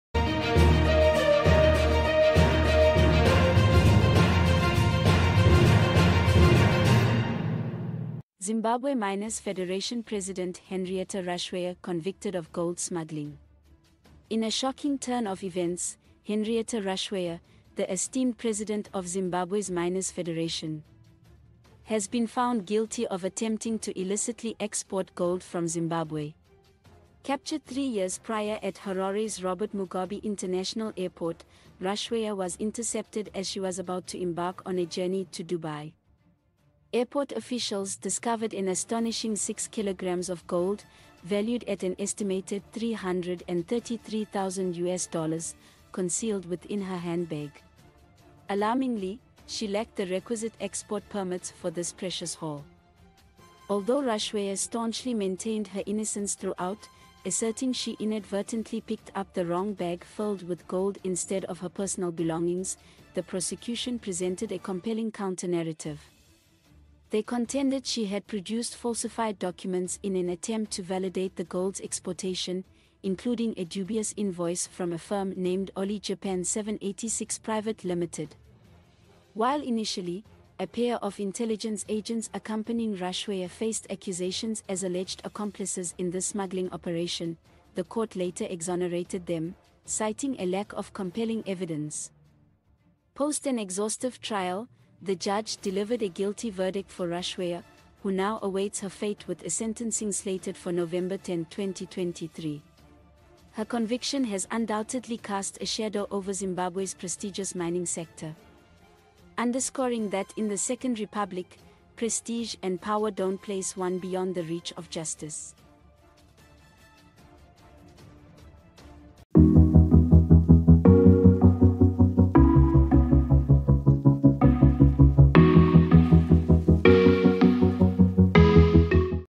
Zimbabwe Miners Federation President Henrietta Rushwaya Conv!cted of Gold Smuggling
In a shocking turn of events, Henrietta Rushwaya, the esteemed president of Zimbabwe's Miners Federation, has been found guilty of attempting to illicitly export gold from Zimbabwe.

Captured three years prior at Harare's Robert Mugabe International Airport, Rushwaya was intercepted as she was about to embark on a journey to Dubai. Airport officials discovered an astonishing 6kg of gold, valued at an estimated US$333,000, concealed within her handbag. Alarmingly, she lacked the requisite export permits for this precious haul.